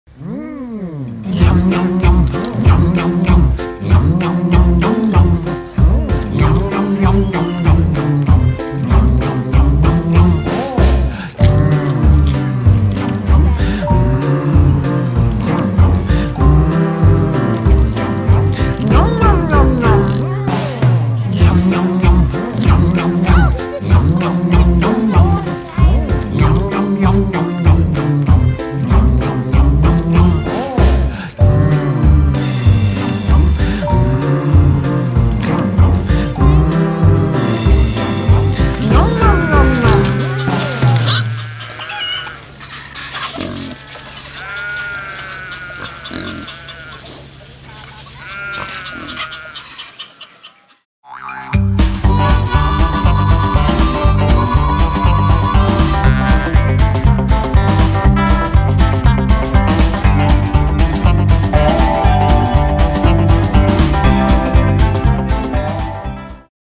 Funny Animal Video!
Gabe and Garrett headed to the local pumpkin patch today and stopped by the petting zoo!  The pigs and sheep and goats and mules were definitely hungry!  The grouchy llama, not so much!  Animals are funny!